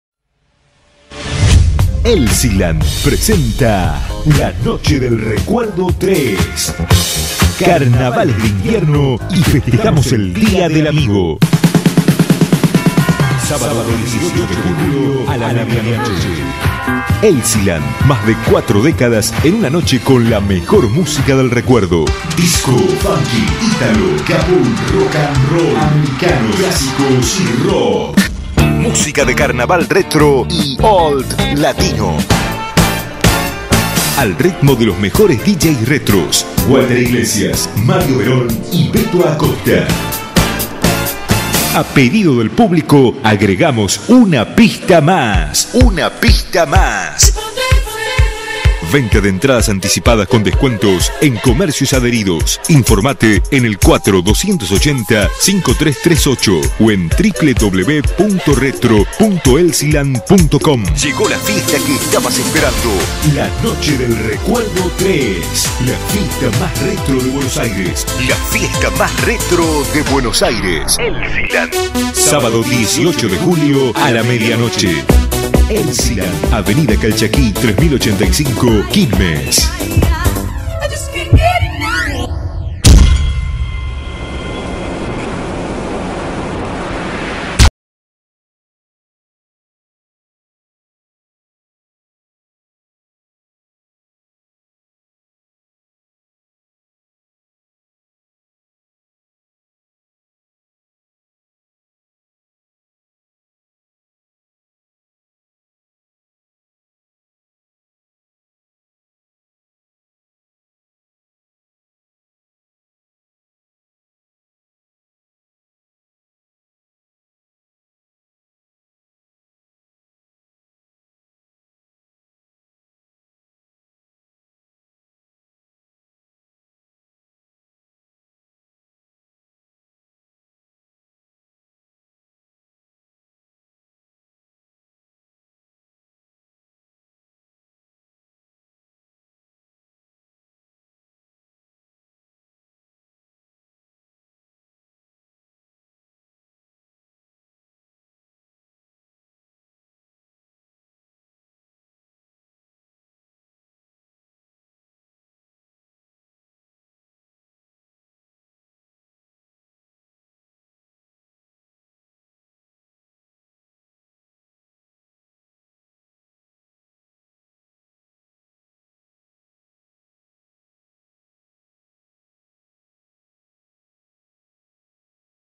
LA FIESTA MAS RETRO DE BS.AS.
Sabado 18 de Julio Elsieland te presenta la 3º fiesta del recuerdo, funk - disco - gapul - italos - pop - americanos rock y clasico
Walter Iglesias - Mario Veron - Beto Acosta
Elsieland - Quilmes - Argentina